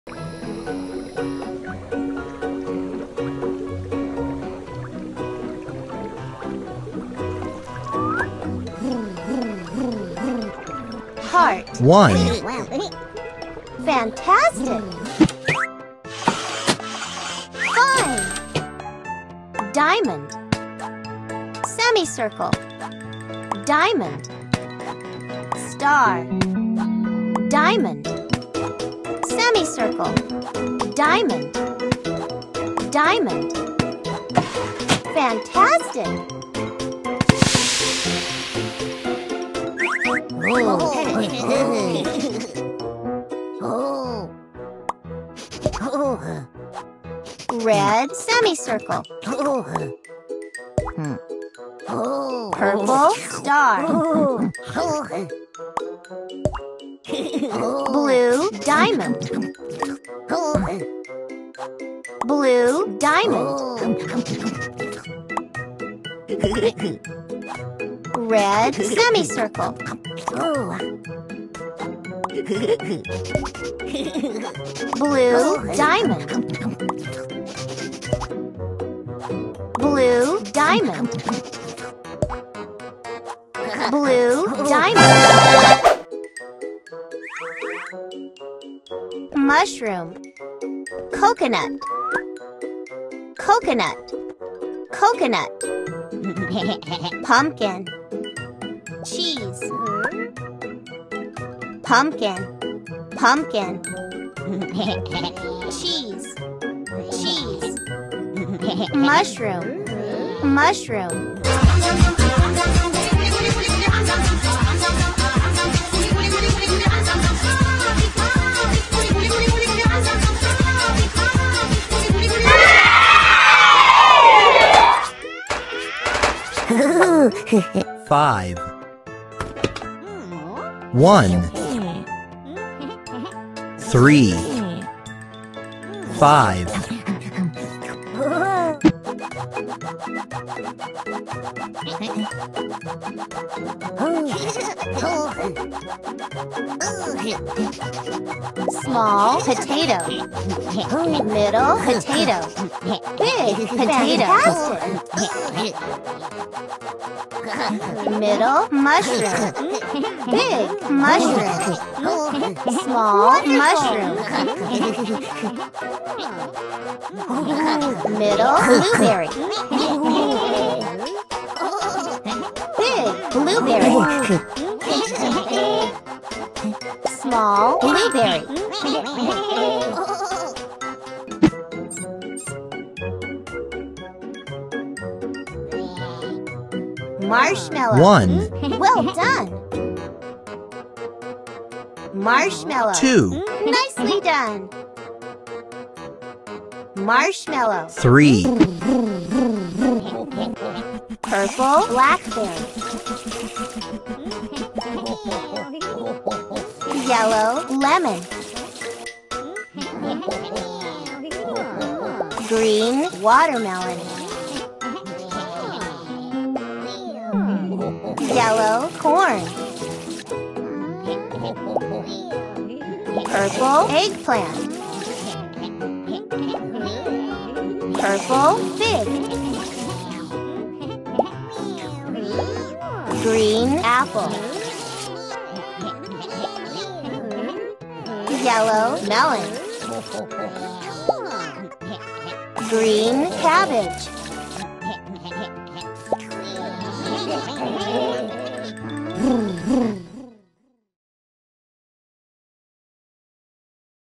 Math for ages 2-4: measurements, logic, shapes, colors
Food fun unleashed! Play with tasty treats in this exciting game.

This cartoon will help your kids to learn shapes, colors, and measurements. It also develops logic and improves attention.

So this video is a perfect way to keep your child occupied while you are preparing dinner.